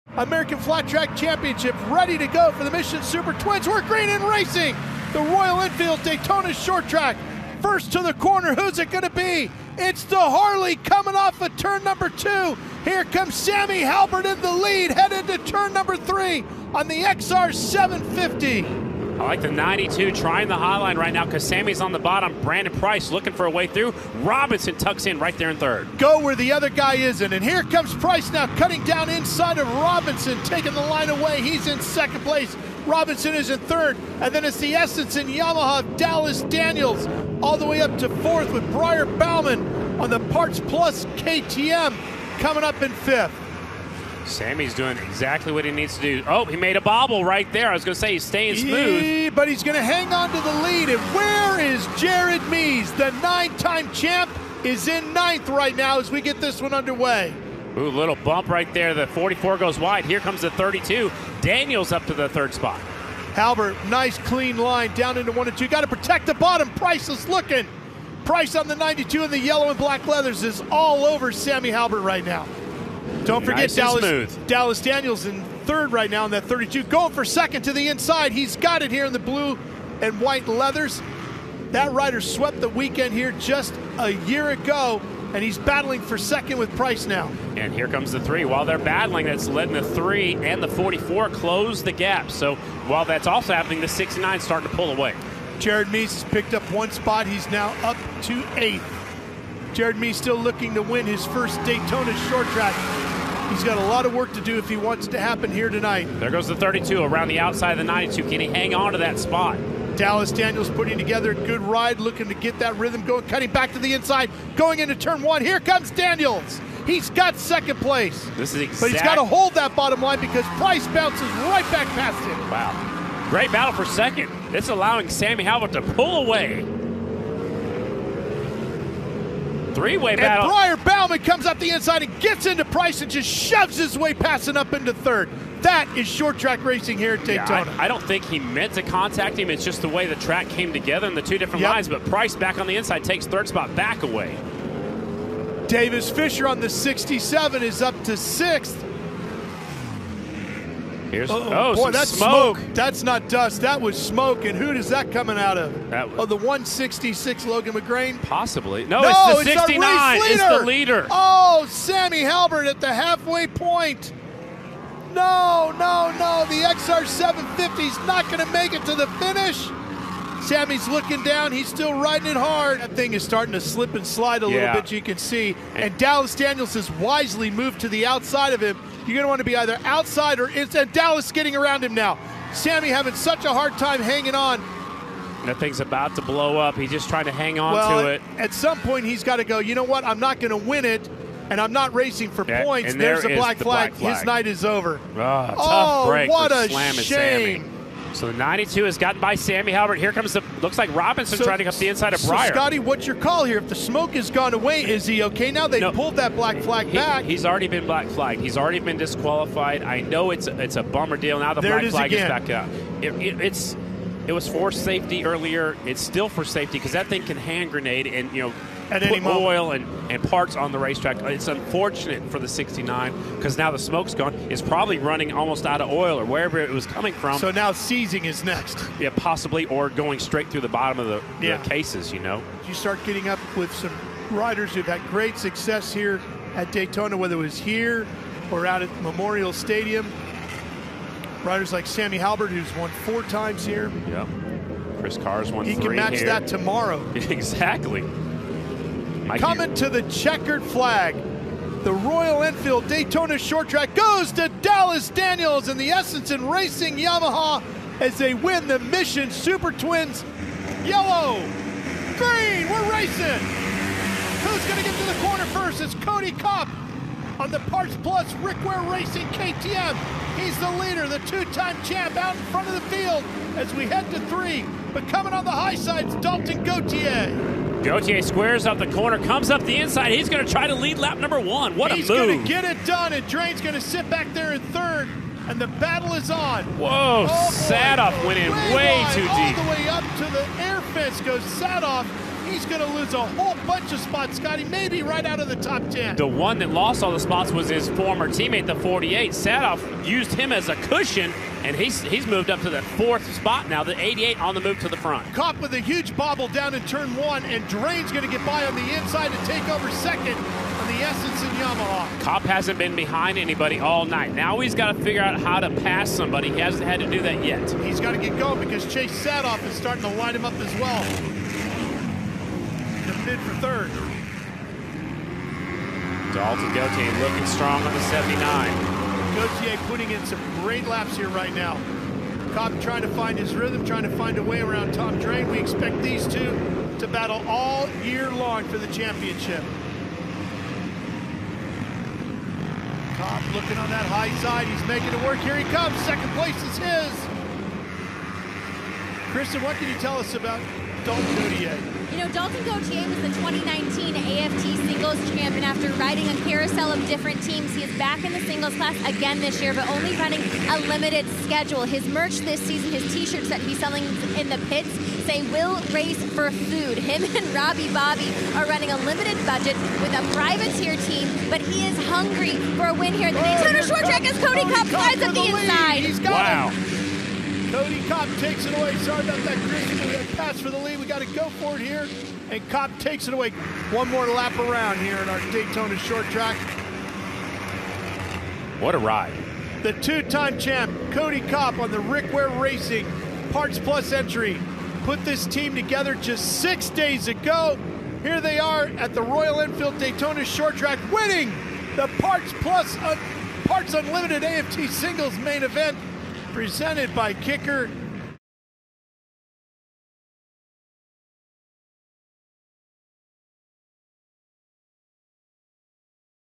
American Flat Track at DAYTONA I 3/7/24 | Highlights
Progressive American Flat Track makes its debut on FloRacing with Royal Enfield Short Track I during Daytona Bike Week on Thursday, March 7, 2024.

Mission SuperTwins and Parts Unlimited AFT Singles compete in a pair of main events to open the season at the DAYTONA Flat Track.

0:00 - AFT SuperTwins Main
4:20-  AFT Singles Main